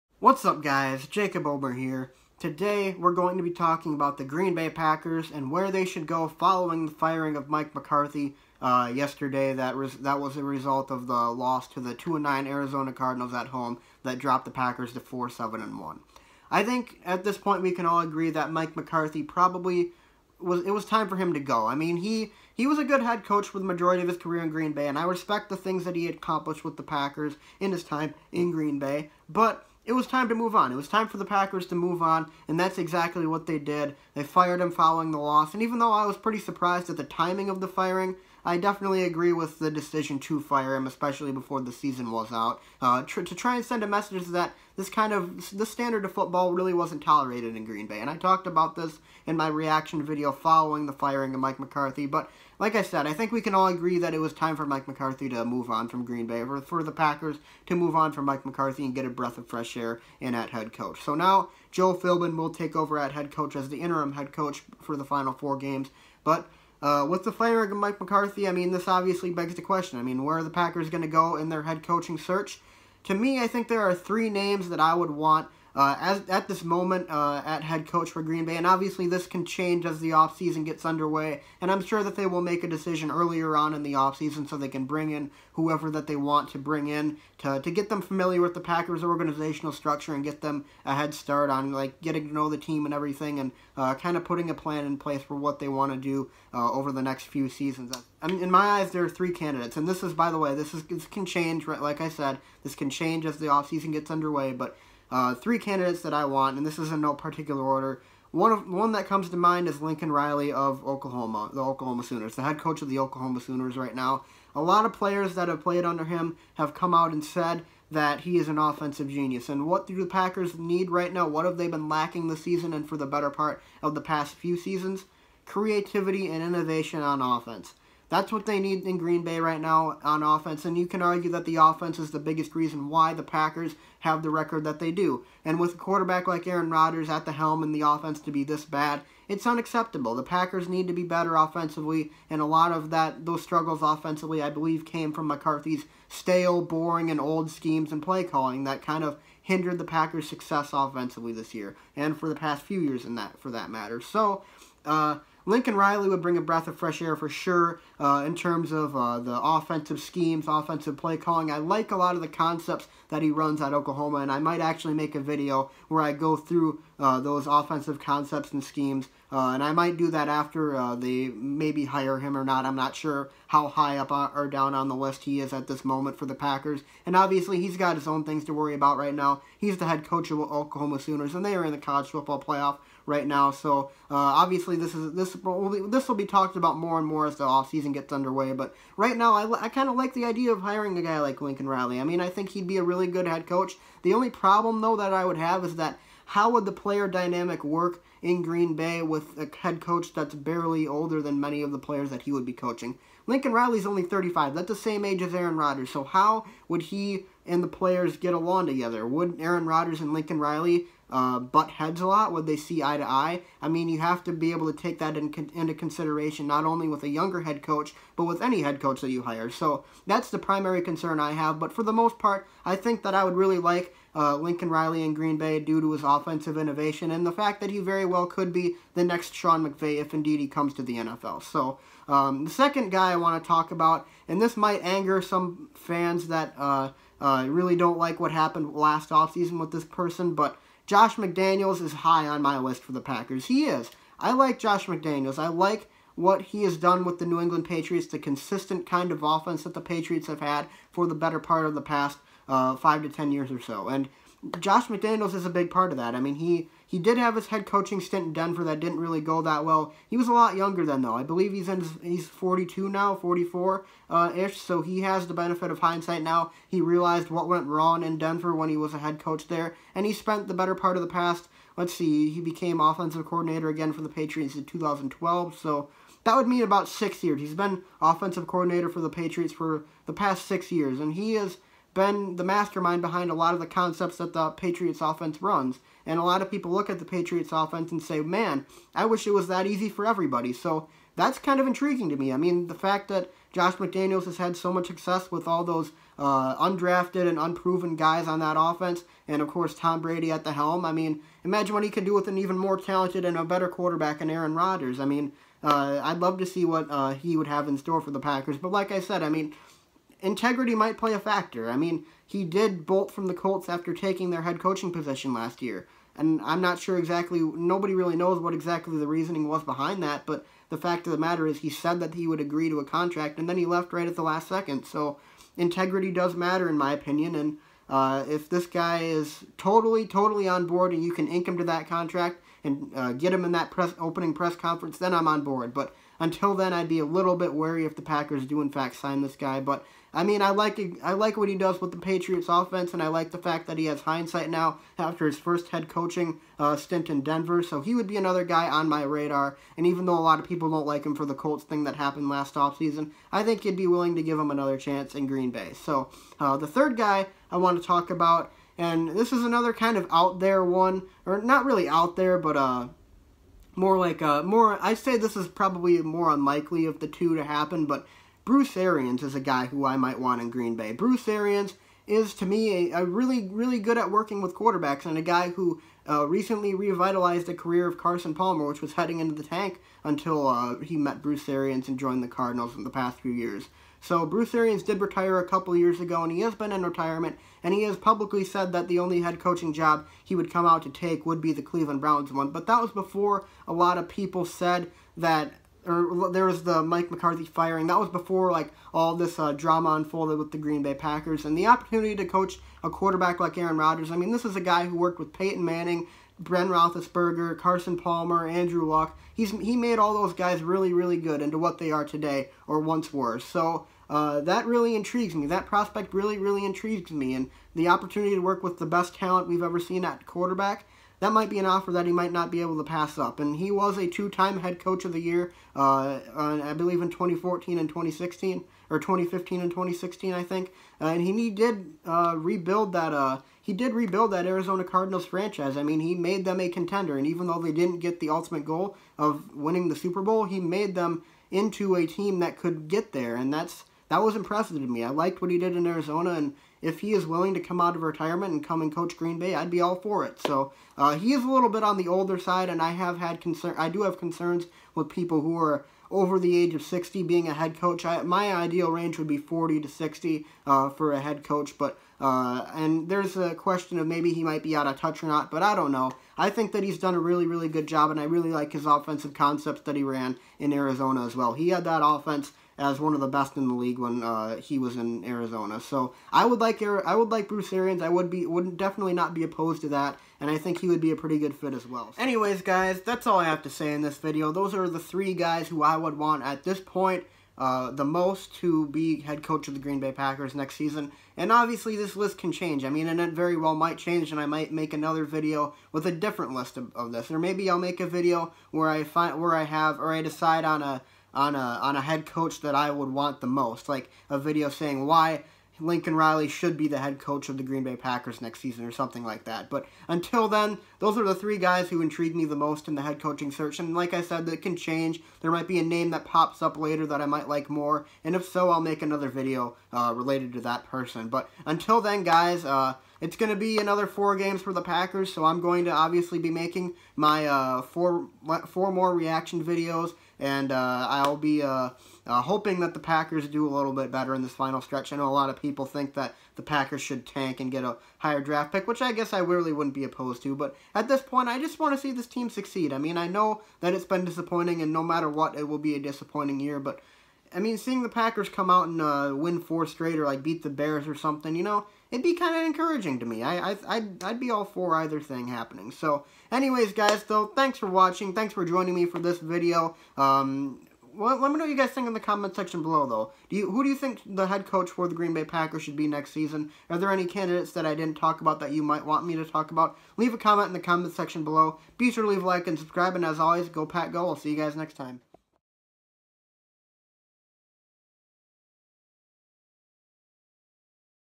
Who Should Be Green Bay’s Next Head Coach?
In this video I give my three most intriguing head coaching candidates for the Packers head coaching position this offseason after the firing of Mike McCarthy. Who do you believe should be the next Packers head coach?